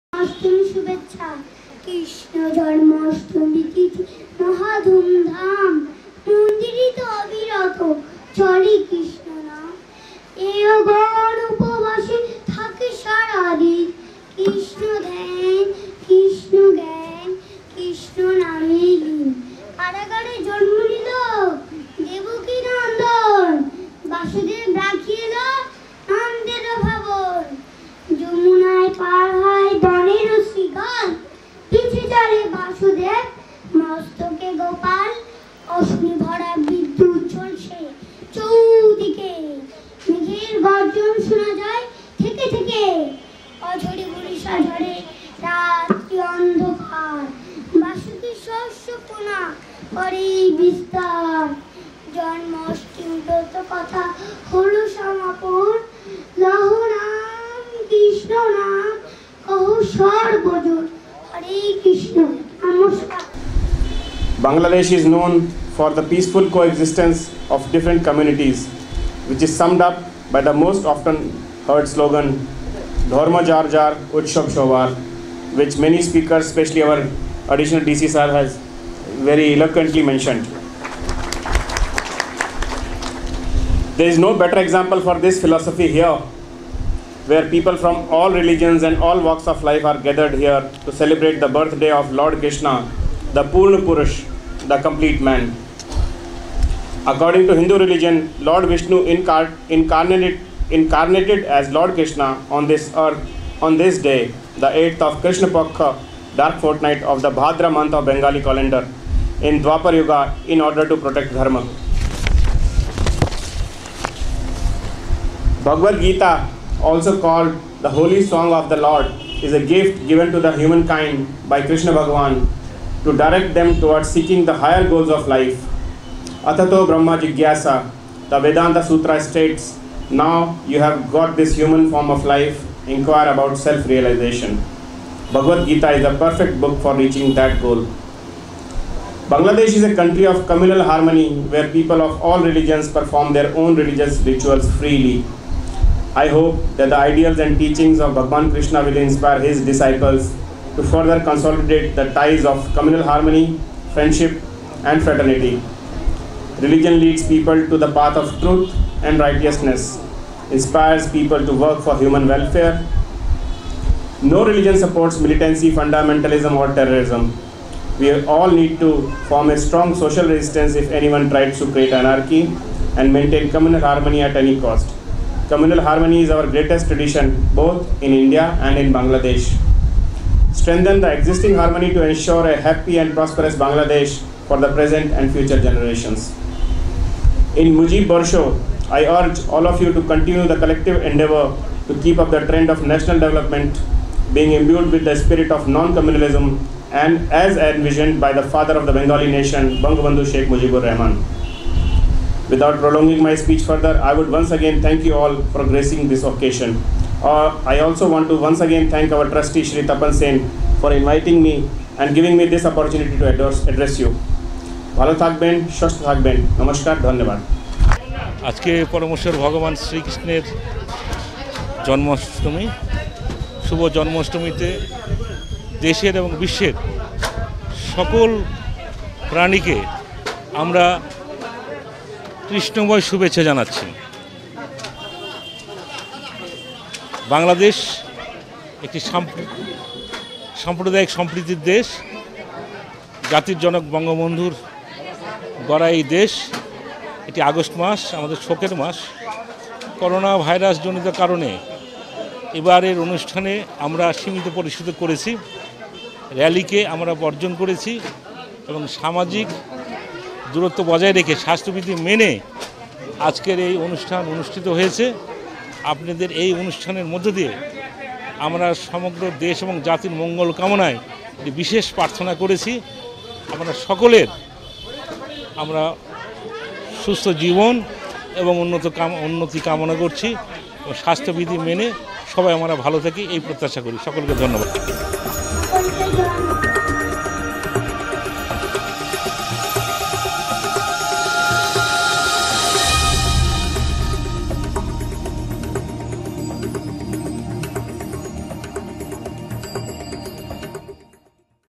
শ্রীকৃষ্ণের জন্মাষ্টমীতে ছোট শিশুর শ্রীকৃষ্নের প্রতি ভক্তি
শুভ জন্মাষ্টমী | সনাতন ধর্মের প্রাণপুরুষ ভগবান শ্রীকৃষ্ণের জন্মদিন, শুভ জন্মাষ্টমী